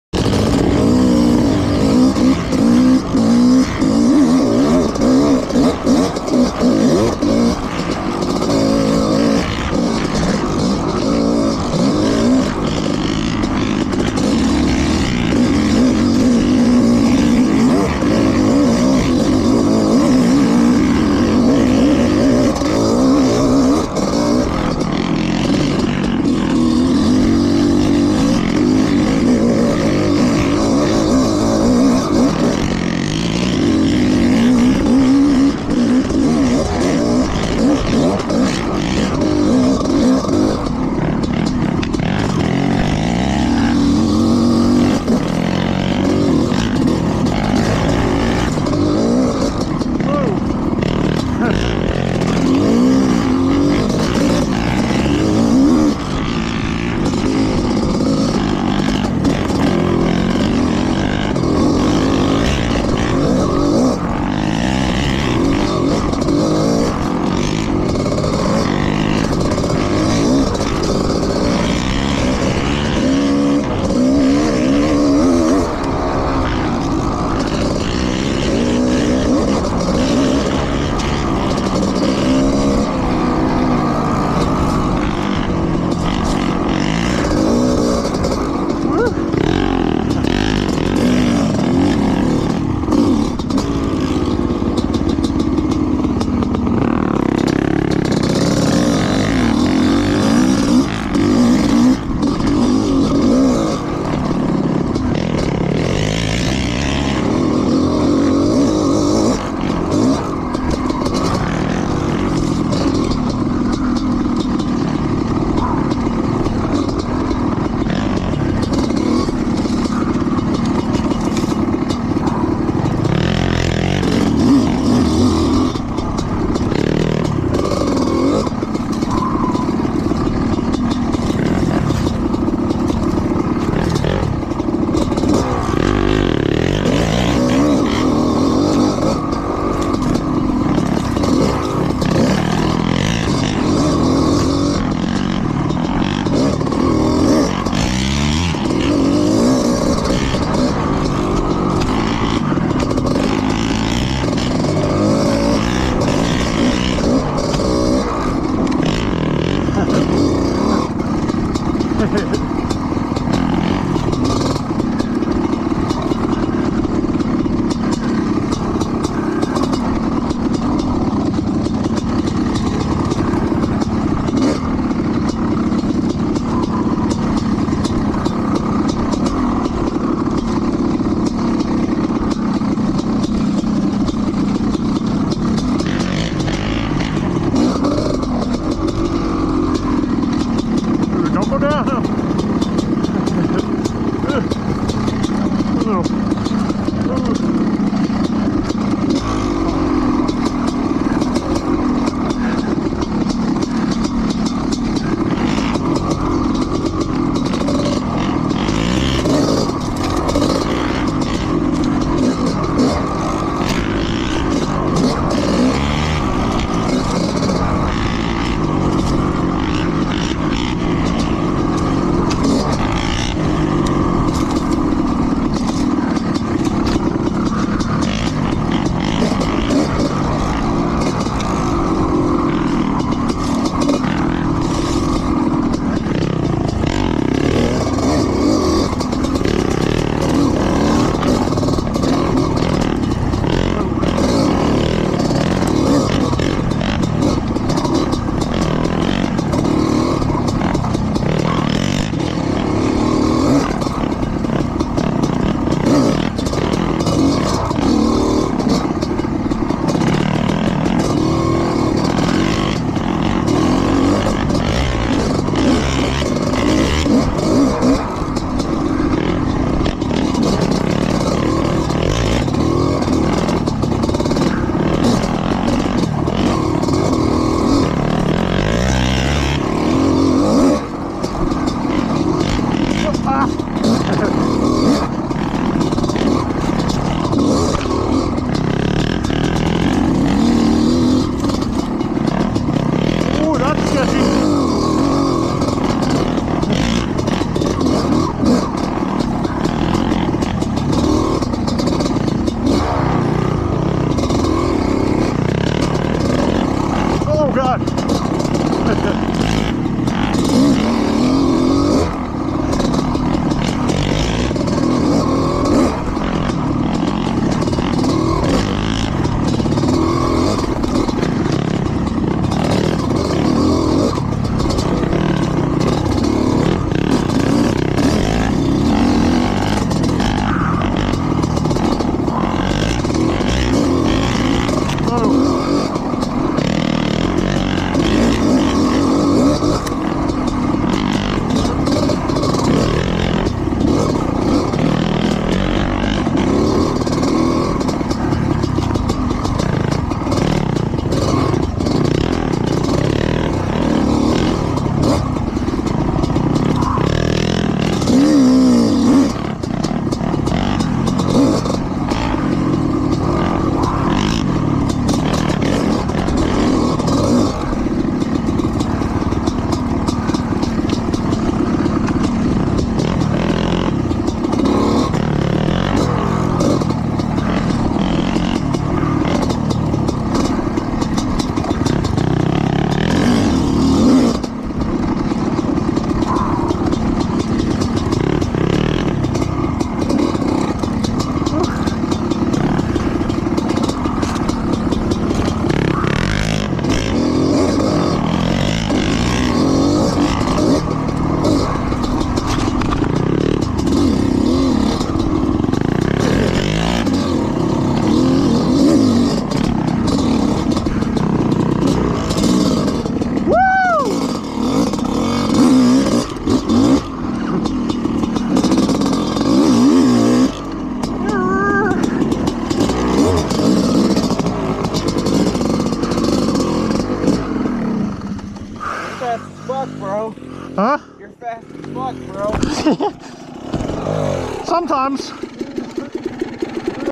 Singletrack Goods
Jammin in Idaho City on the Ratbike, 94 Honda CR500.